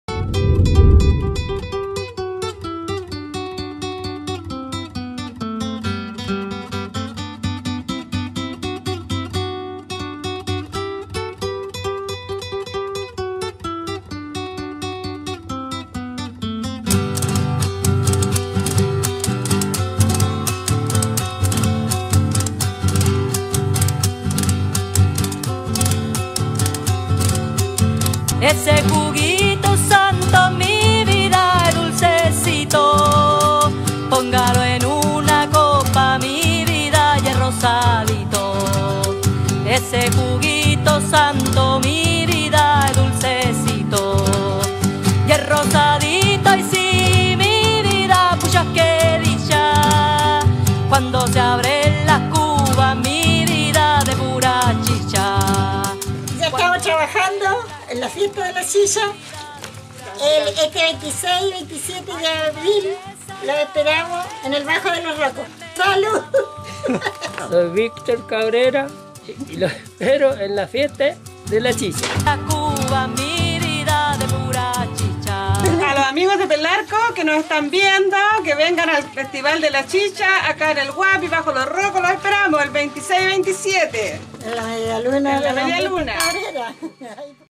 Fiesta de la Chicha en Bajo Los Rocos Pelarco
Te invitamos a disfrutar de la Primera Fiesta de la Chicha, en bajo Los Rocos, Comuna de Pelarco 26 y 27 de Abril, Pelarco invita.